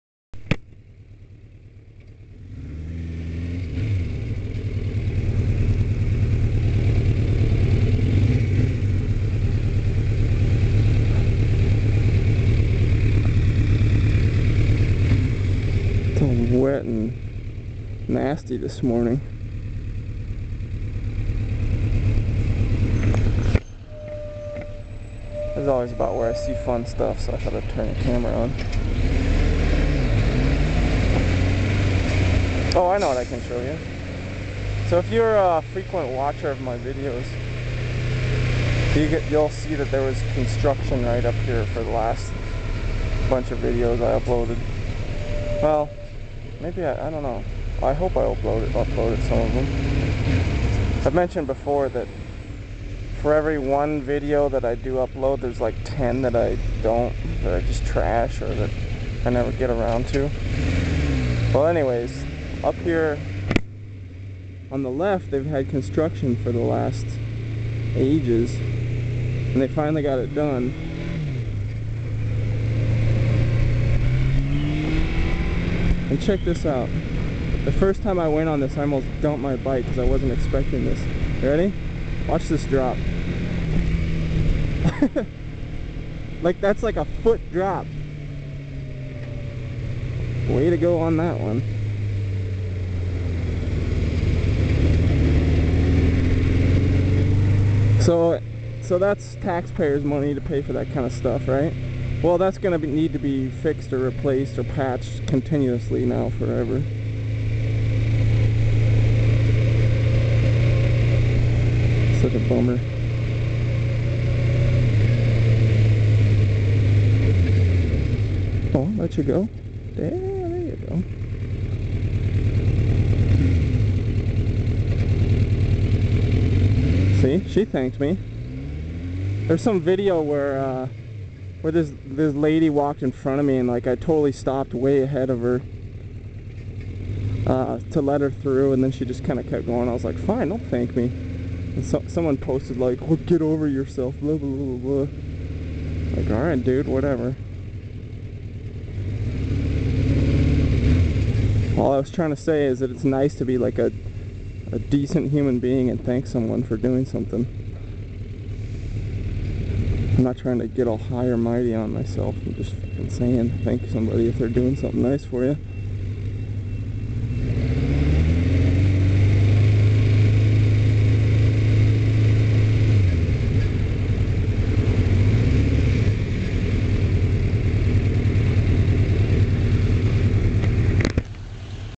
jabbering away
just kinda chatin about stuff in general, nothing to exciting. i wanted to film because it was rainy and poor conditions, never know what might happen!